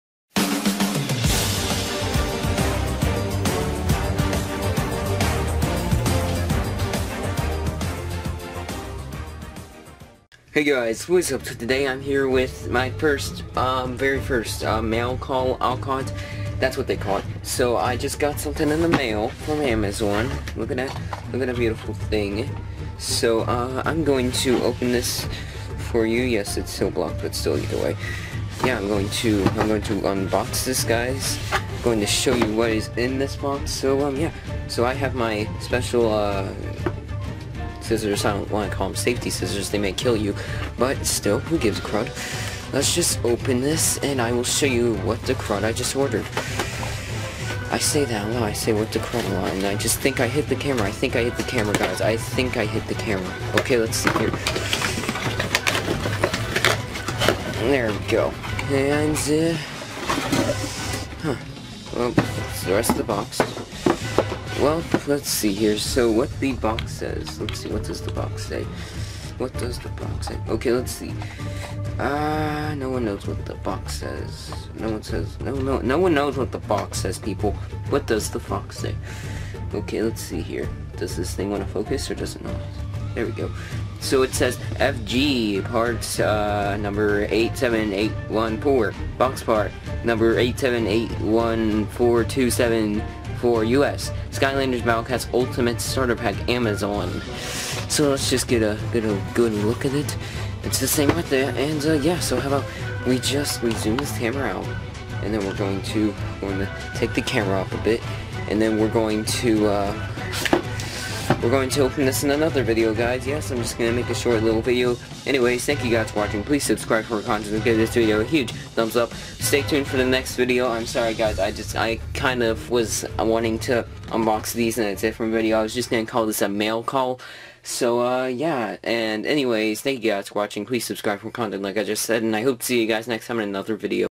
Skylanders Battlecast Mail Call #1
► Hello and Welcome to Fire Fiesta my fellow Fiestalings! Today, I show you what I just got in the mail :P

► Sign the petition to bring back battle mode (for Skylanders Six): Search "Skylanders Battle Mode petition" on Google for petition :)
► Gamecenter(s) - Fire Fiesta! and Cute Platyp
us Perry